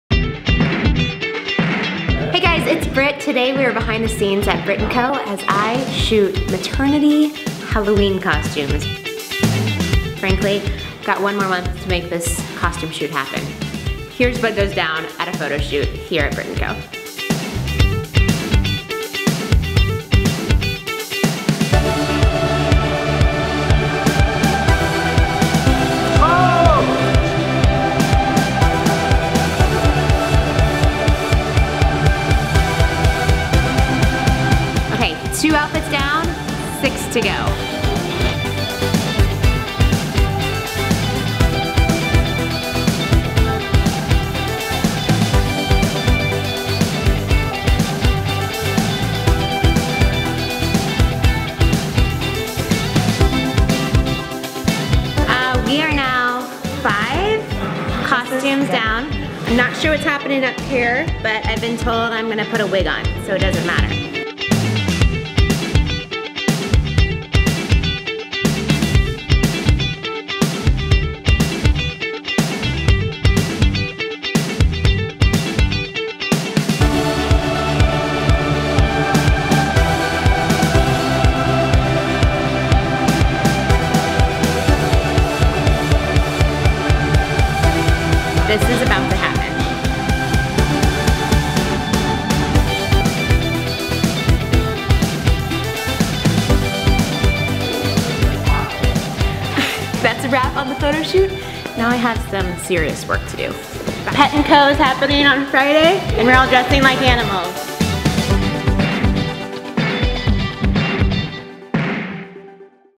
Brit IRL: Behind the Scenes at a B+C Photo Shoot
It's never too early to start thinking about Halloween... Come behind the scenes with Brit as she models costumes for our fave holiday!